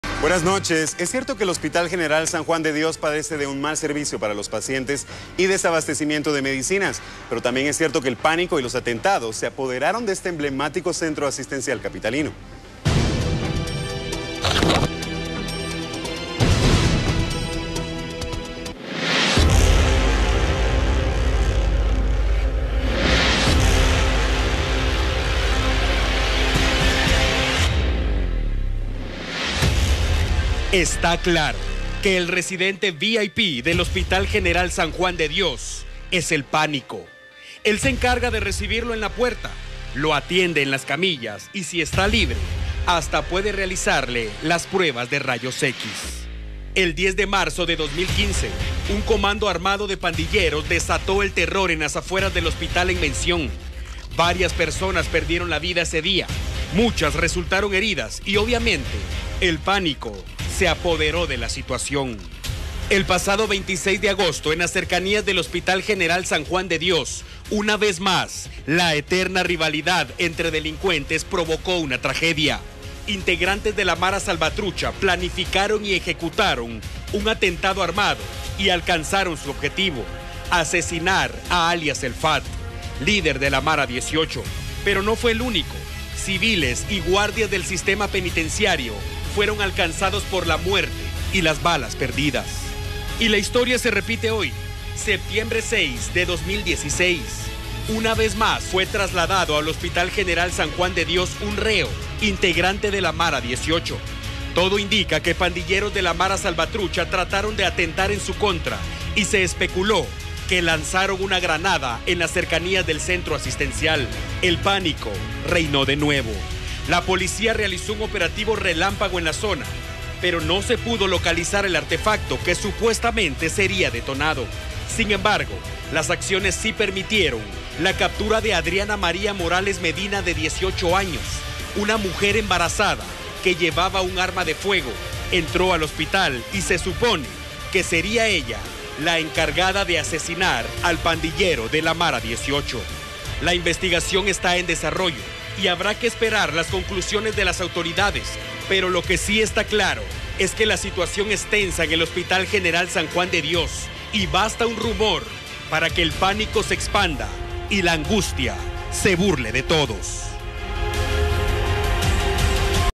Atentados armados en cercanías del Hospital San Juan de Dios han sido fatales
Es cierto que el Hospital San Juan de Dios padece de un mal servicio para los pacientes y desabastecimiento de medicinas, pero también es cierto que los atentados y el pánico se apoderaron del centro médico. Un rumor, un operativo, una mujer capturada y pánico generalizado en hospital.